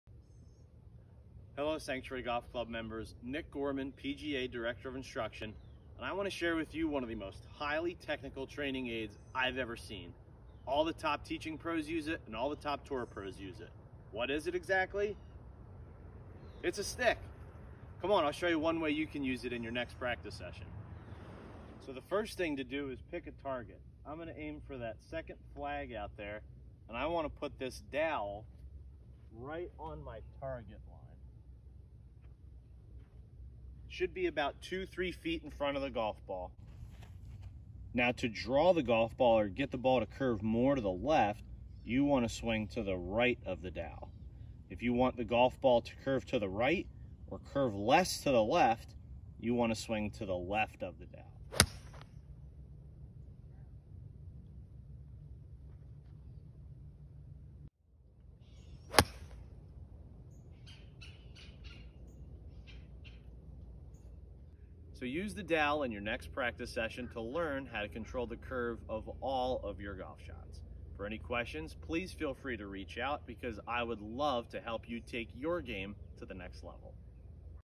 How to measure the curvature of your golf shots in practice
How to use a dowel in practice to measure your swing path.